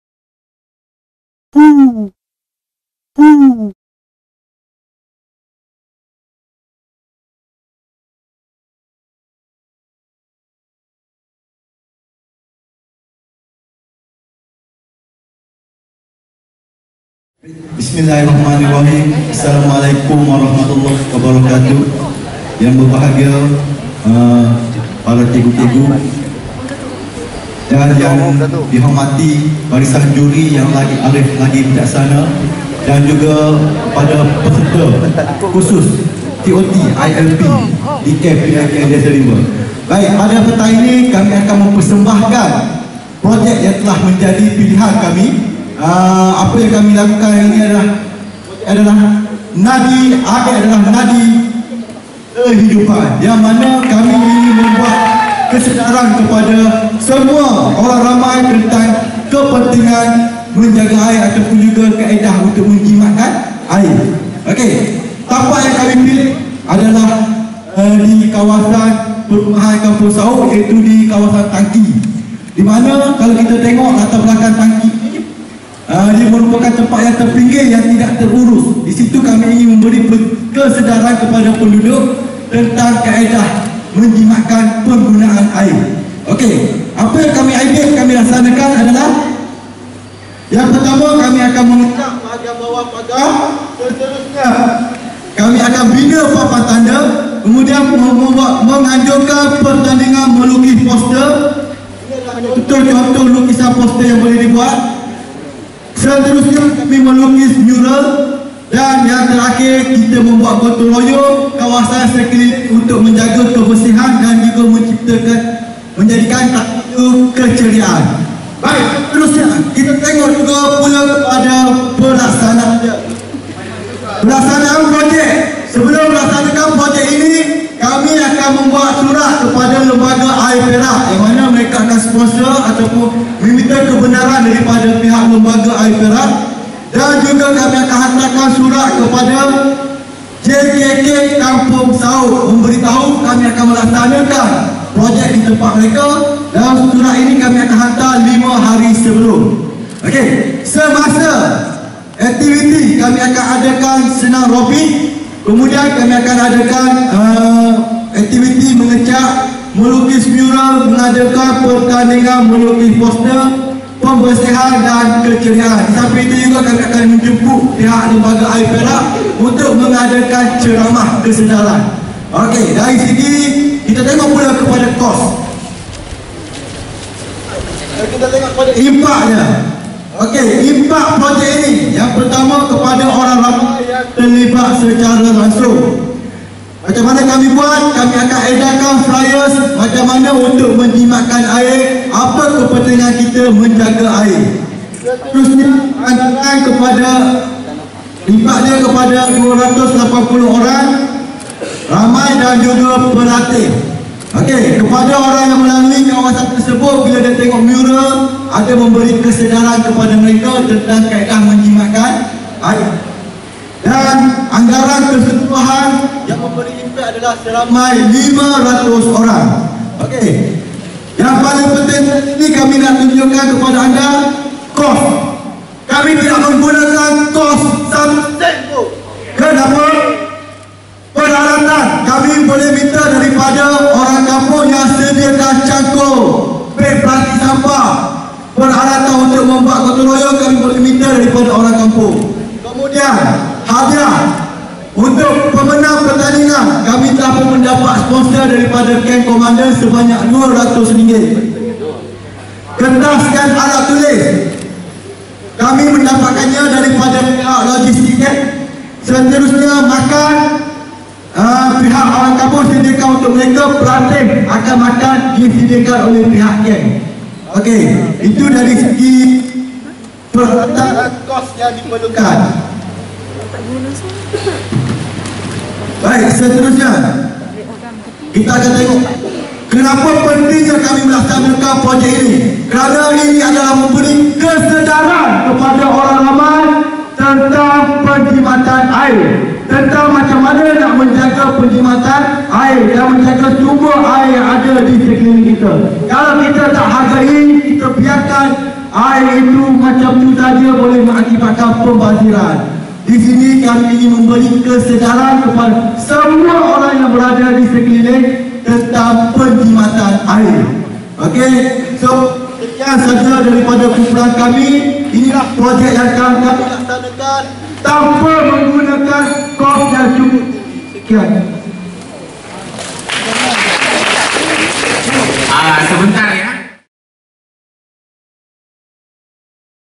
PMI: Projek Air Nadi Kehidupan (PLKN TOT di Kem Desa Rimba)
Peserta TOT i-Upgrade dan i-Serve Siri 2/2016 di Kem PLKN Desa Rimba, Chenderoh Perak membentangkan projek AIR NADI KEHIDUPAN semasa sessi Pitch My Idea. Dari 6 projek yang dibentangkan3 projek telah dipilih oleh Juri yang terdiri dari wakil-wakil Jurulatih dari beberapa kem di Zon A, B dan G

TOT PLKN2.0 untuk Modul I-Upgrade dan I-Serve ini telah dilaksanakan dari 24 Feb hinge 8 Mac 2016 melibatkan 157 orang jurulatih dari zon terbabit